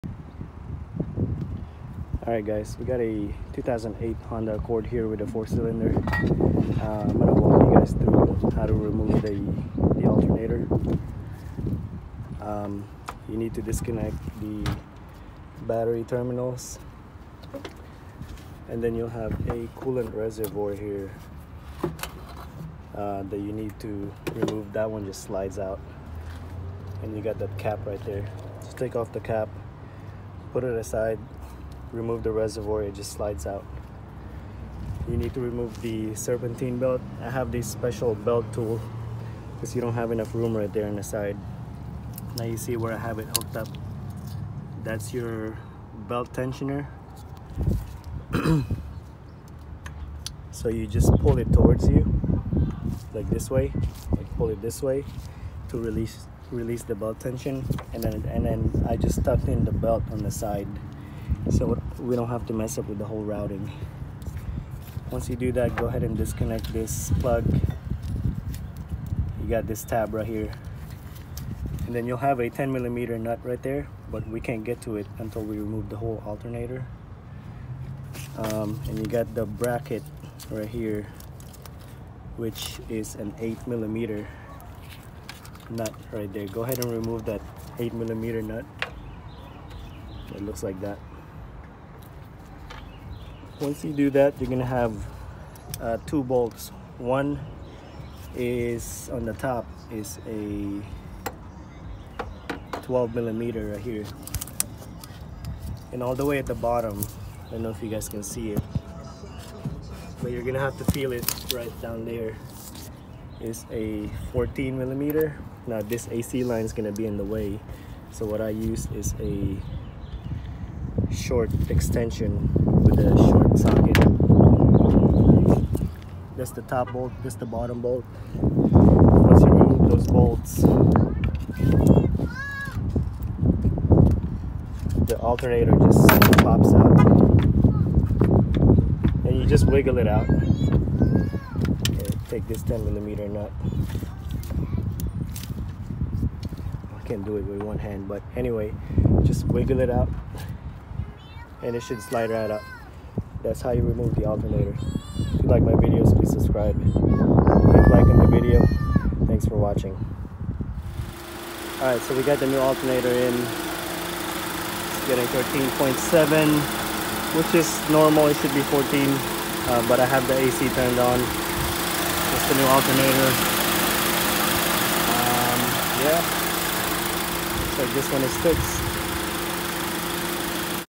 How to remove 2008 - 2012 Honda Accord 2.4L Alternator
How to remove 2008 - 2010 Honda Accord Alternator with a 2.4L engine.